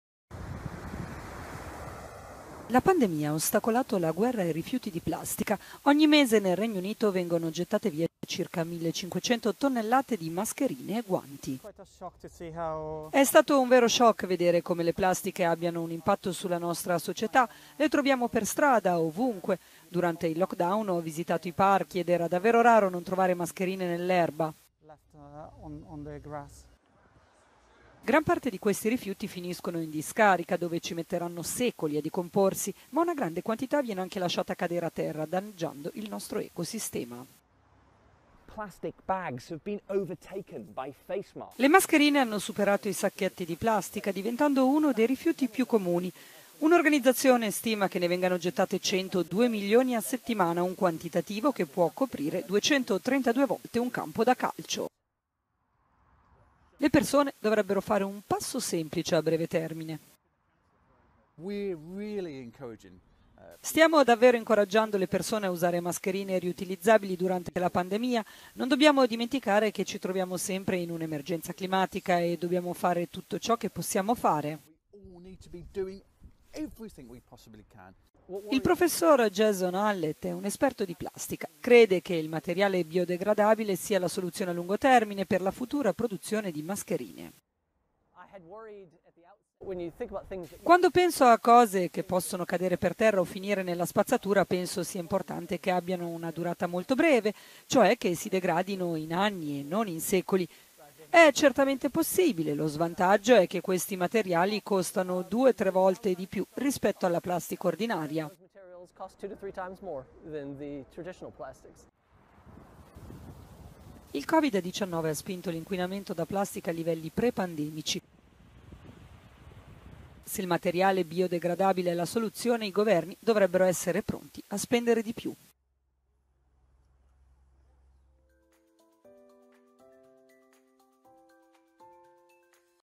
Pandemia e inquinamento: la lotta ai rifiuti di plastica
Una volta erano le buste di plastica il rifiuto più comune, oggi invece queste sono state rimpiazzate dalle mascherine. Nel Regno Unito ad esempio vengono gettate circa 150 tonnellate tra guanti e mascherine, molti di questi rifiuti finiscono nelle discariche, ma altri vengono gettati in terra finendo per inquinare in nostro ecosistema